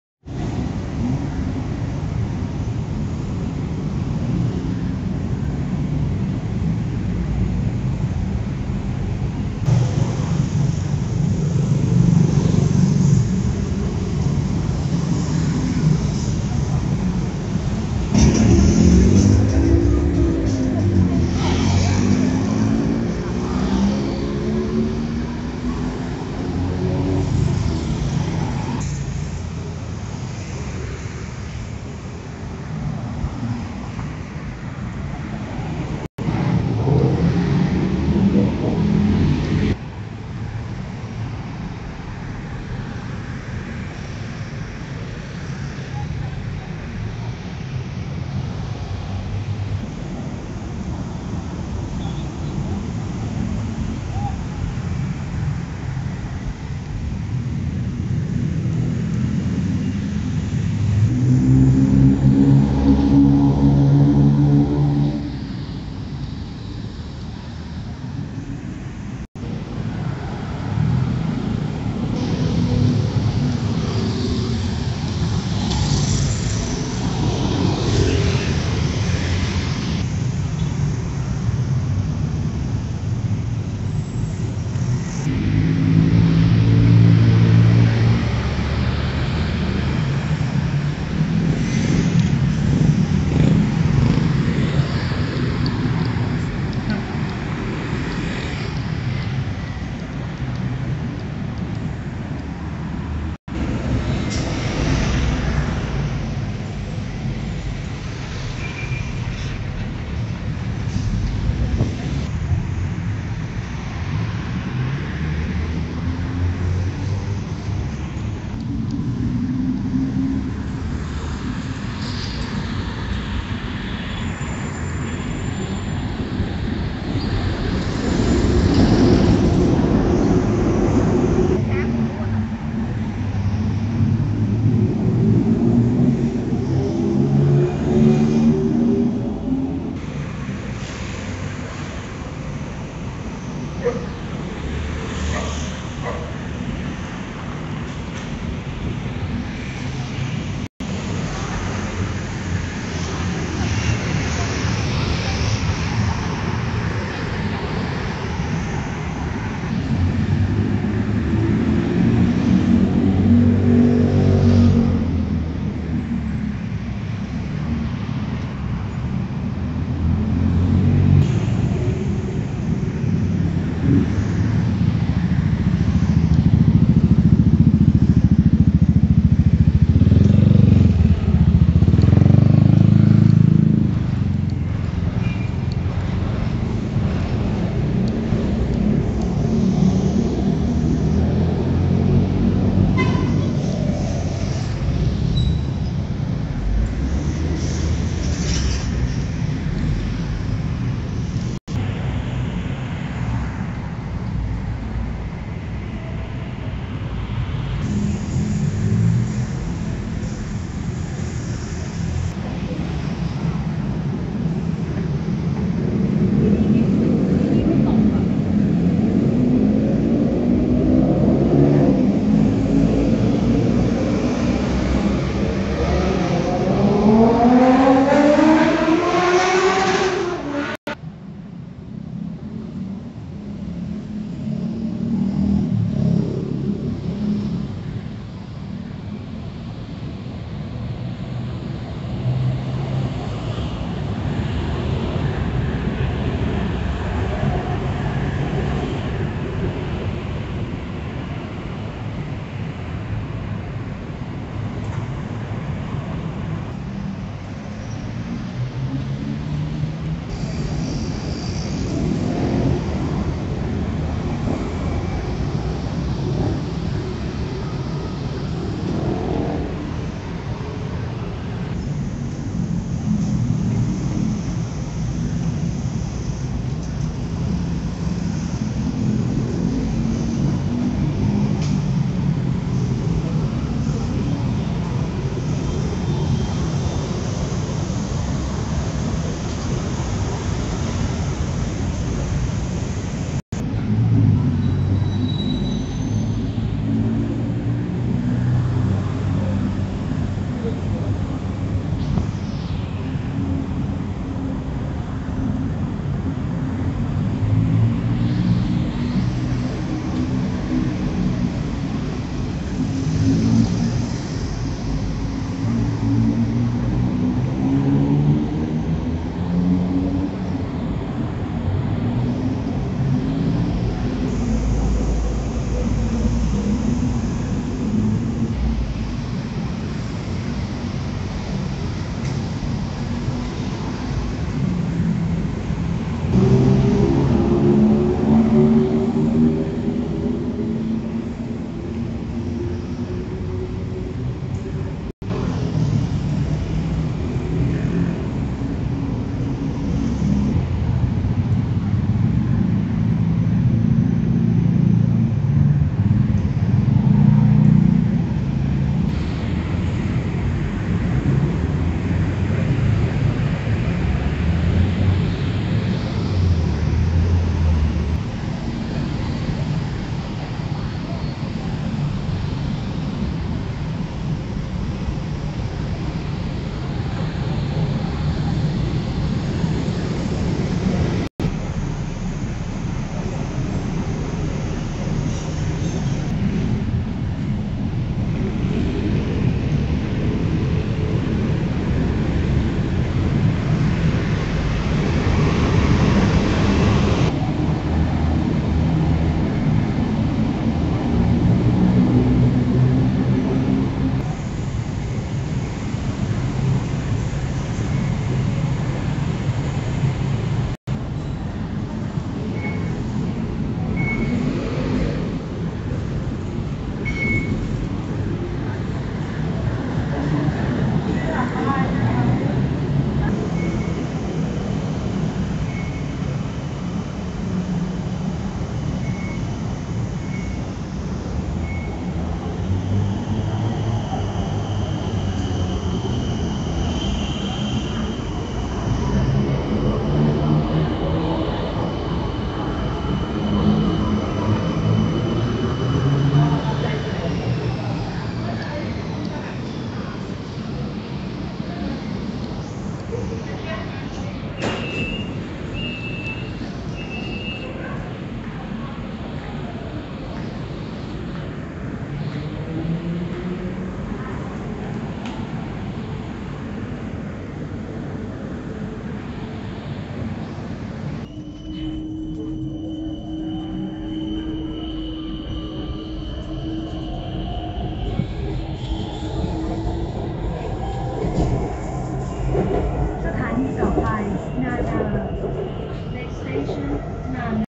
The intersection of Asoke, Sukhumvit,Bangkok,Thailand at night, the lights are very beautiful.
Take a walk around the intersection of Asoke, Sukhumvit, Bangkok, Thailand at night, the lights are very beautiful.